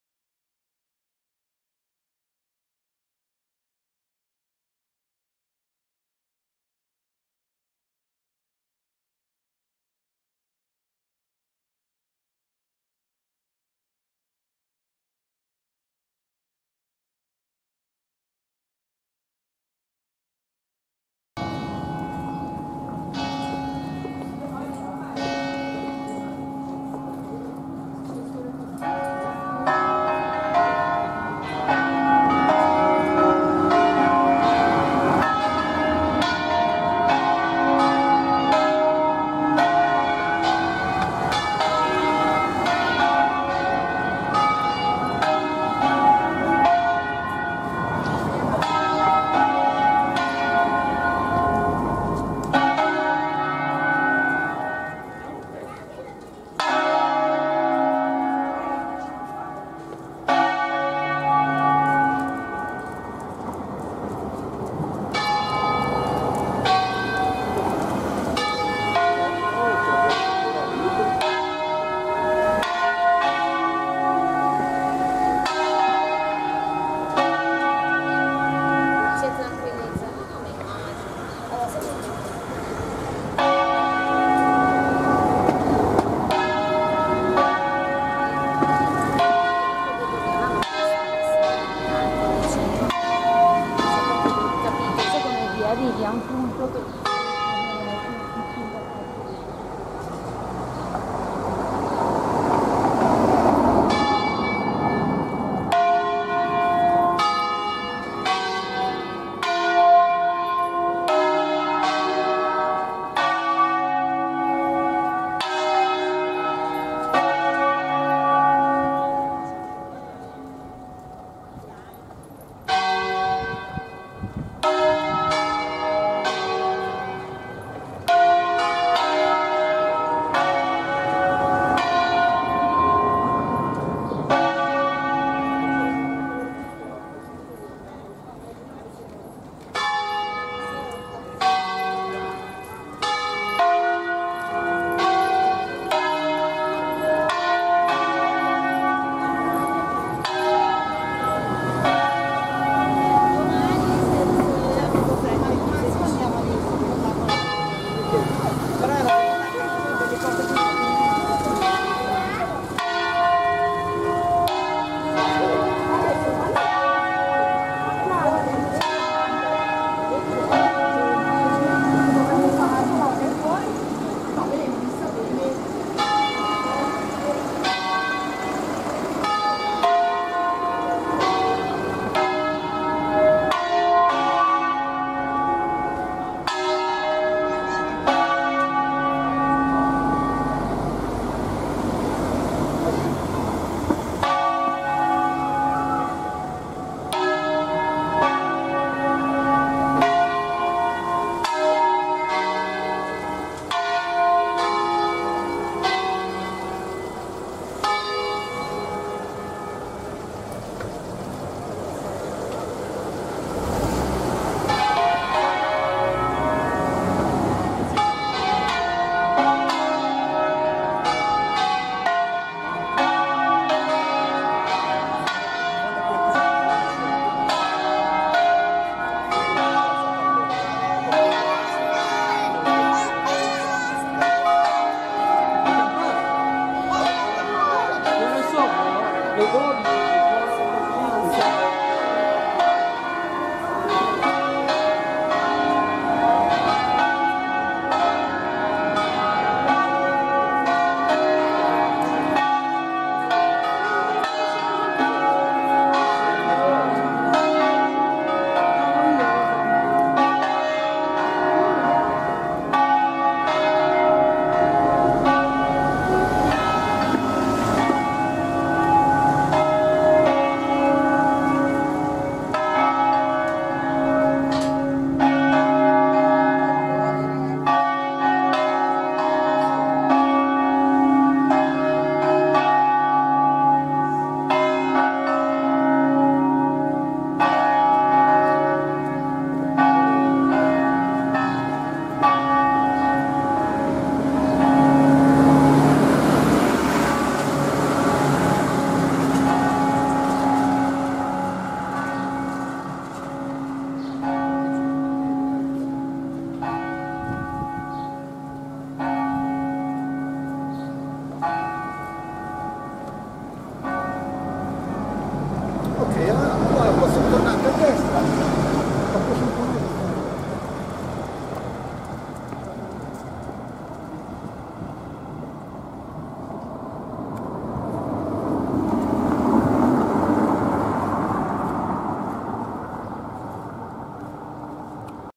Ferragosto 2022, parte 2 - le campane di Ponte di Legno (BS)
Ponte di Legno (BS)
Chiesa parrocchiale della Santissima Trinità

Concerto di 5 campane in Reb3 crescente
Giorgio Pruneri - Grosio
Solb3 rif. Francesco d'Adda - Crema
Reb3 e Mib3 rif. Capanni - Castelnuovo ne' Monti 1996

Concerto solenne per l'Angelus festivo nella Solennità dell'Assunzione di Maria. Ore 12

Lunedì 15/08/2022